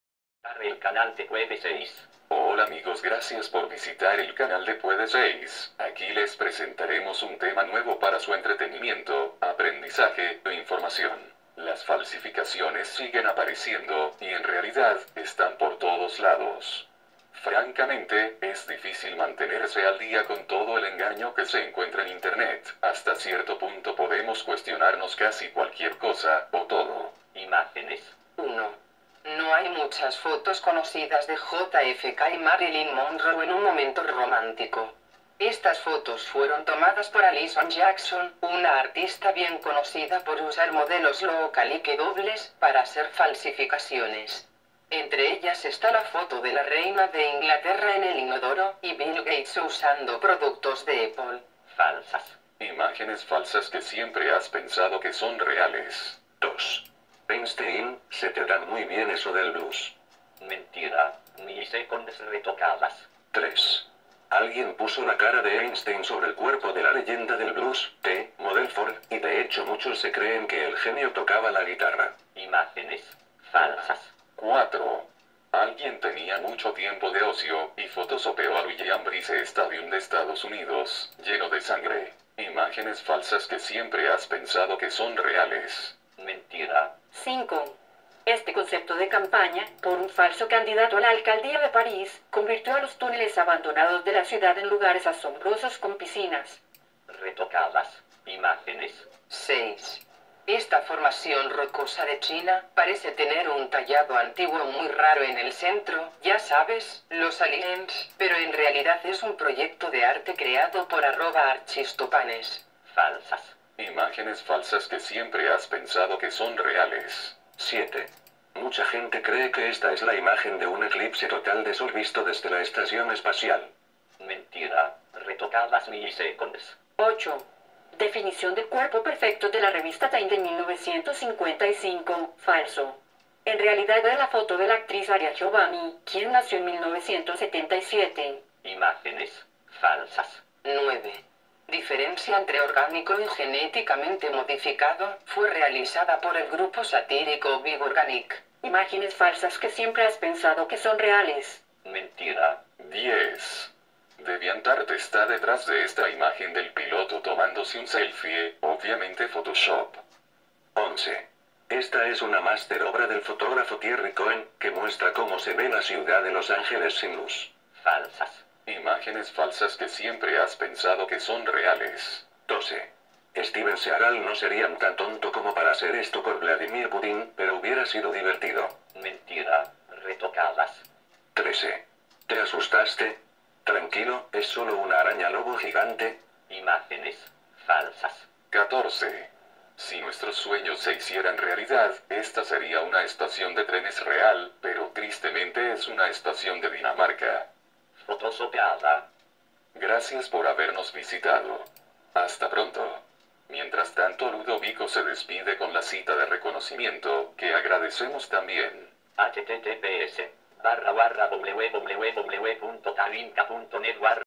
Imágenes falsas que siempre has pensado que son reales
Las falsificaciones siguen apareciendo, y en realidad, están por todos lados. Francamente, es difícil mantenerse al día con todo el engaño que se encuentra en Internet... Hasta cierto punto podemos cuestionarnos casi cualquier cosa... O TODO.